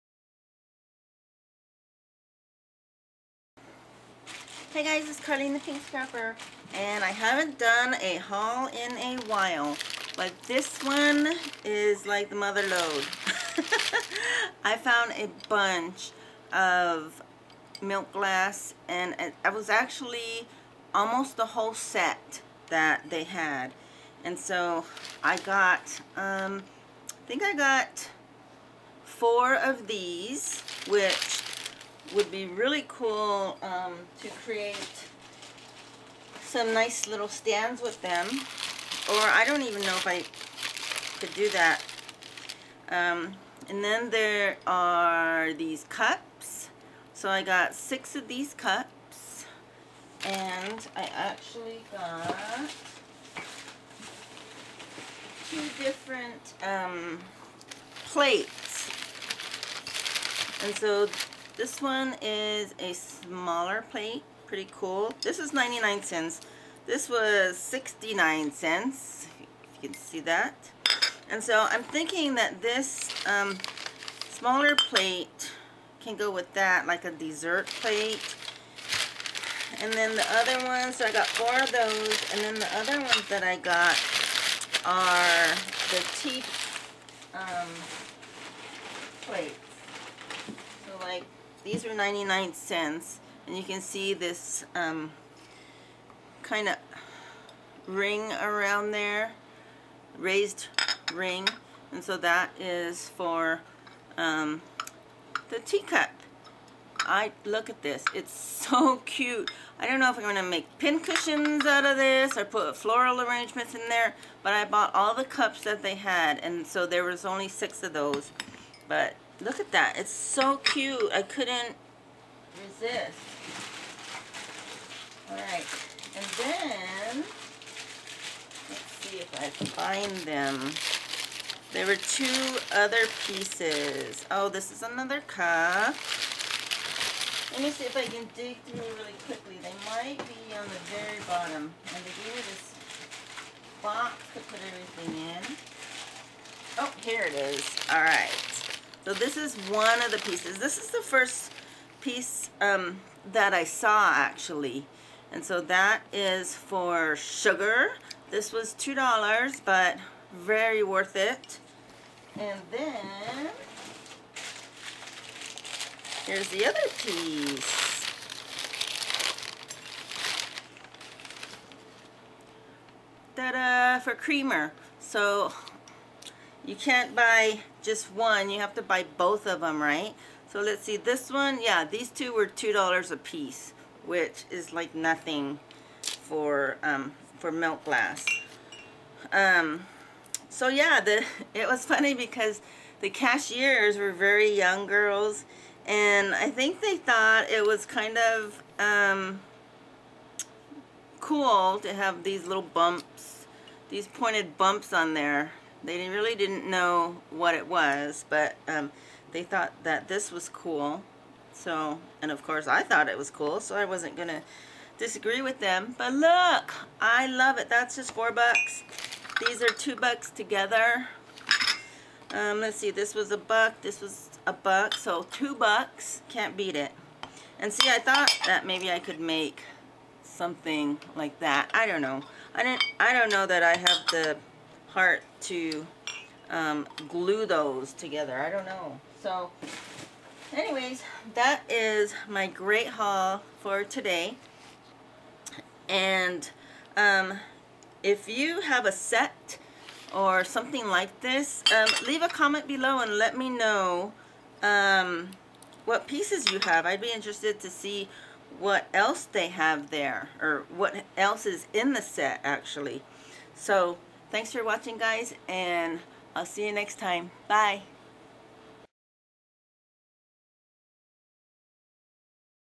Small Thrift Store haul
Check out this small but mighty thrift store haul.  Comment if you have any ideas what I should create with it ! I'd love to hear it.